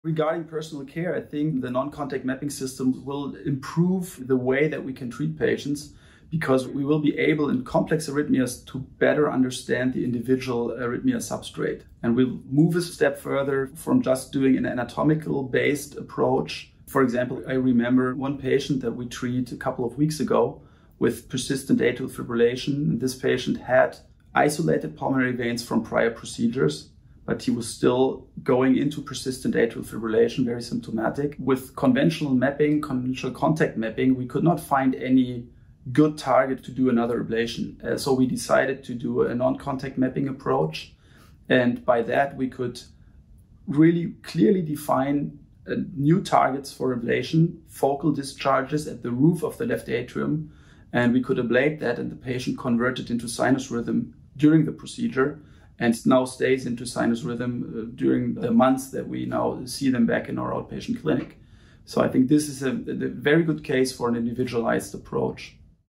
Individualized Therapy - Prof. Dr. Boldt, Charité Berlin
Prof. Dr. Boldt from Charité Berlin explains why it is important to have the right tools and technology to efficiently and effectively treat patients with complex and unstable arrhythmias.